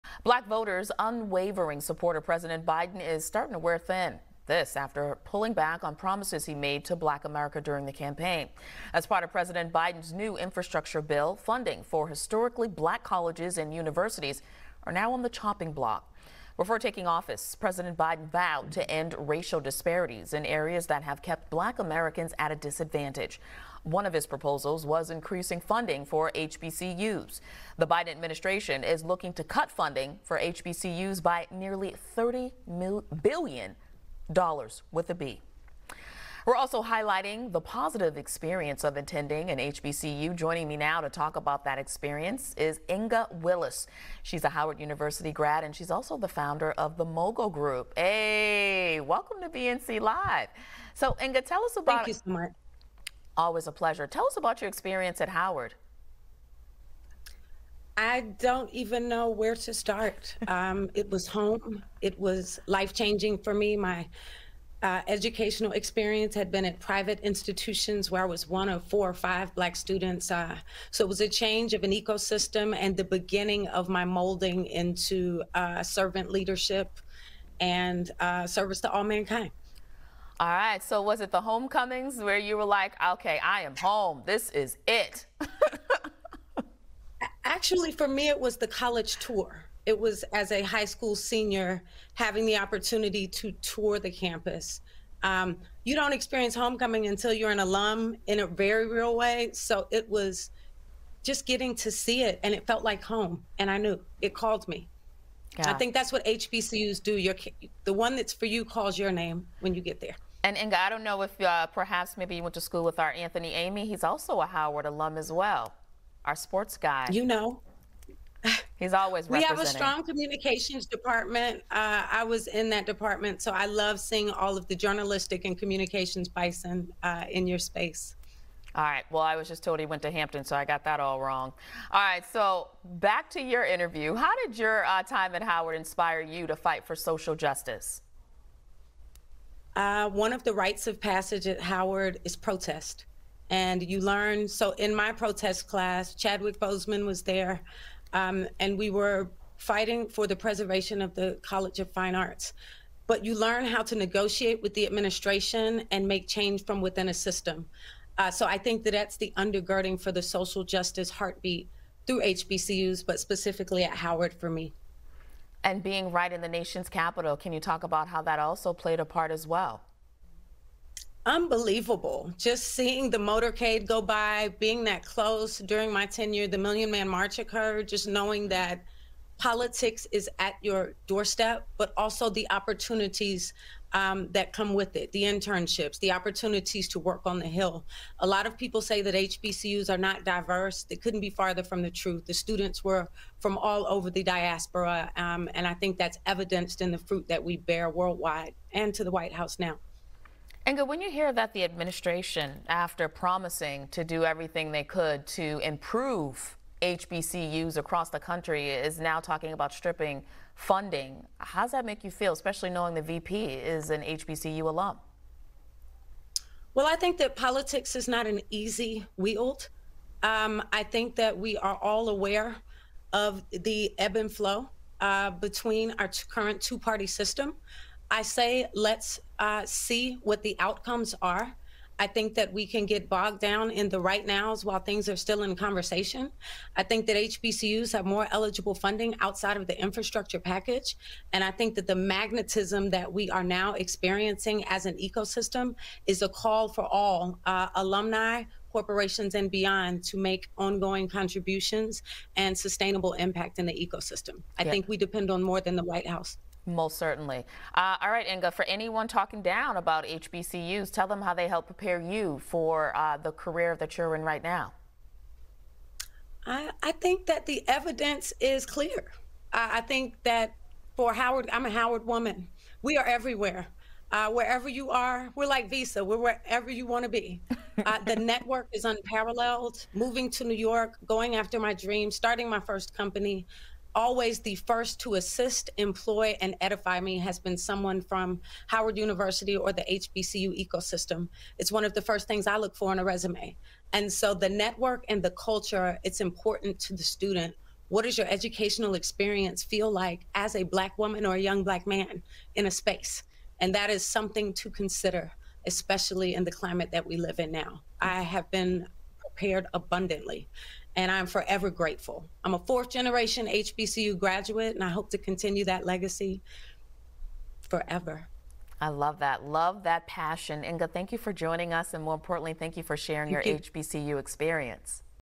Highlighting the HBCU Experience, Howard Alum Recounts Time on The Hill
As part of President Biden's new infrastructure bill, funding for historically Black colleges and universities are now on the chopping block. Before taking office, President Biden vowed to end racial disparities in areas that have kept Black Americans at a disadvantage. One of his proposals was increasing funding for HBCUs. Now, the Biden administration is looking to cut funding for HBCUs by nearly $30 billion.

Howard University alum and founder of the Mogul Group, Inga Willis, joins “BNC Live” with Tashanea Whitlow to discuss her experience with her HBCU, and she works to propel her fellow graduates in the working world. She also speaks on how her exposure to HBCU life at a young age from relatives made her choice an easy one.